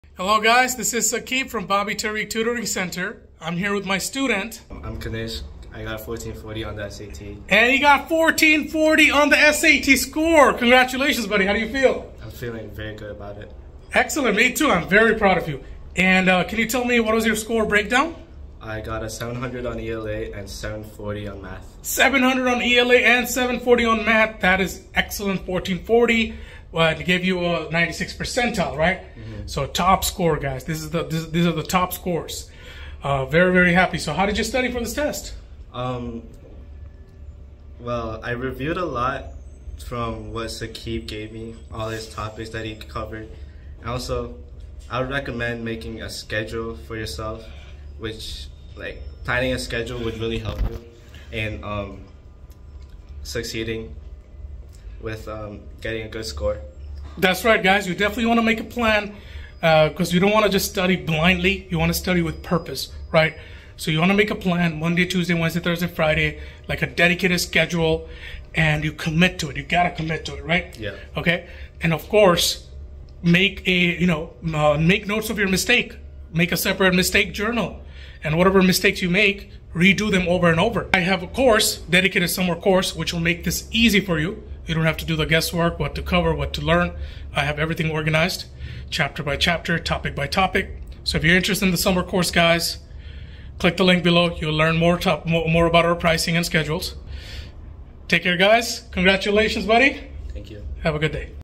10th Grader scores 1440 on Digital SAT | Top 4% Nationwide | Bobby-Tariq Tutoring Center
My student, Kanishk, a 10th grader, scored 1440 on the Digital SAT. This score places him in the top 4% of all test-takers. Every year, only 3% to 7% of students out of nearly 2 million test-takers achieve this score!

Score Breakdown:
Reading & Writing: 700/800
Math: 740/800
Total: 1440/1600

By:
Sakib Hussain
CEO & SAT/SHSAT Instructor